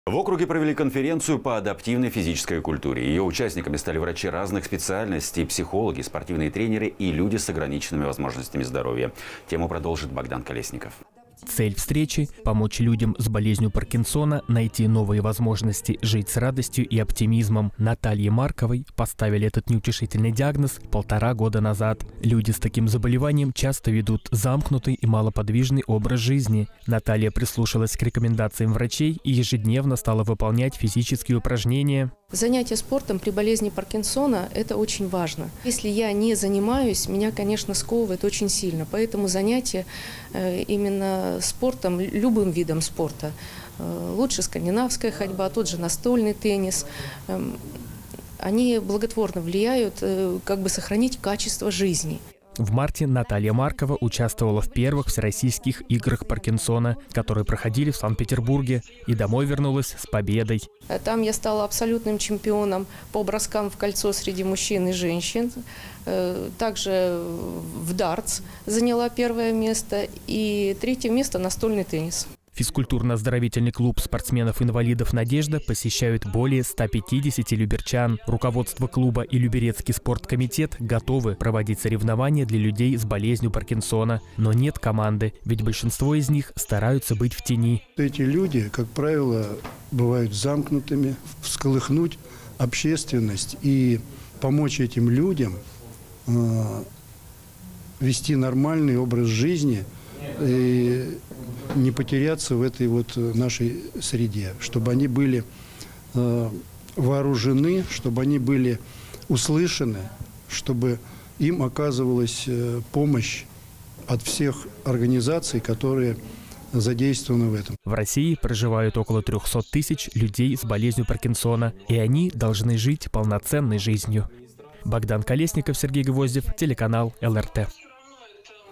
Движение - это жизнь
В округе провели конференцию по адаптивной физической культуре. Её участниками стали врачи разных специальностей, психологи, спортивные тренеры и люди с ограниченными возможностями здоровья.

Цель встречи: помочь людям с болезнью Паркинсона найти новые возможности жить с радостью и оптимизмом. Наталье Марковой поставили этот неутешительный диагноз полтора года назад. Люди с таким заболеванием часто ведут замкнутый и малоподвижный образ жизни. Наталья прислушалась к рекомендациям врачей и ежедневно стала выполнять физические упражнения.

Наталья Маркова, участница физкультурно-оздоровительного клуба спортсменов-инвалидов «Надежда» г.о. Люберцы:

«Занятия спортом при болезни Паркинсона очень важны. Если я не занимаюсь, меня, конечно, сковывает очень сильно, поэтому занятия любым видом спорта, лучше скандинавская ходьба или тот же настольный теннис, они благотворно влияют на сохранение качества жизни».

В марте Наталья Маркова участвовала в первых Всероссийских играх Паркинсона, которые проходили в Санкт-Петербурге. И домой вернулась с победой.

«Там я стала абсолютным чемпионом по броскам в кольцо среди мужчин и женщин. Также в дартс заняла первое место. И 3-е место – настольный теннис».

Физкультурно-оздоровительный клуб спортсменов-инвалидов «Надежда» посещают более 150 люберчан. Руководство клуба и люберецкий спорткомитет готовы проводить соревнования для людей с болезнью Паркинсона. Но нет команды. Ведь большинство из них стараются быть в тени.

Юрий Грязнов, начальник отдела по адаптивной физической культуре Дирекции спортивных сооружений г.о. Люберцы:

«Эти люди, как правило, бывают замкнутыми. Всколыхнуть общественность и помочь этим людям вести нормальный образ жизни и не потеряться в нашей среде. Чтобы они были вооружены, были услышаны, чтобы им оказывалась помощь от всех организаций, которые задействованы в этом».

В России проживают около 300 тысяч людей с болезнью Паркинсона. И они должны жить полноценной жизнью.